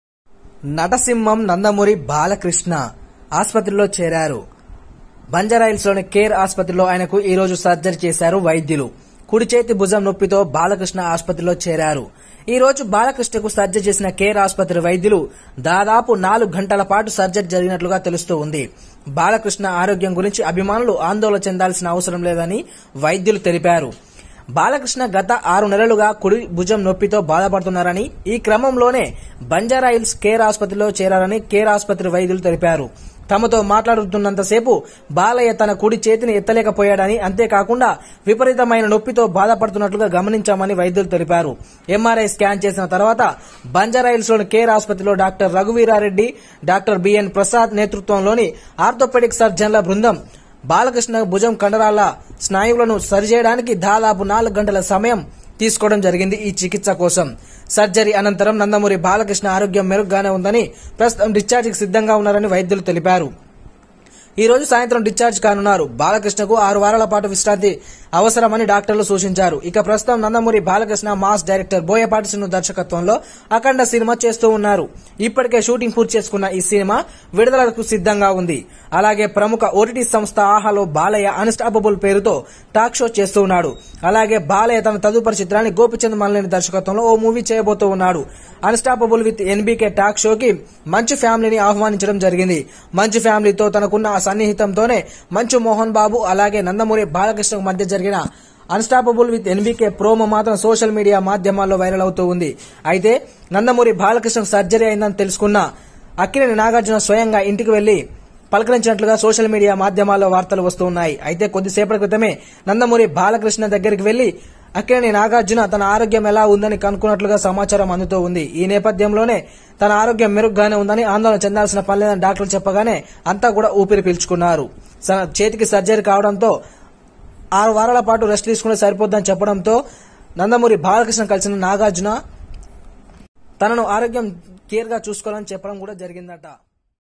కేర్ హాస్పిటల్ లో చేరిన బాలయ్య ను పరామర్శించడానికి హుటాహుటిన వెళ్లిన నాగార్జున | Balayya Care
కేర్ హాస్పిటల్ లో చేరిన బాలయ్య ను పరామర్శించడానికి హుటాహుటిన వెళ్లిన నాగార్జున | Balayya Care Hospital Latest News || Tollywood Film Garage ||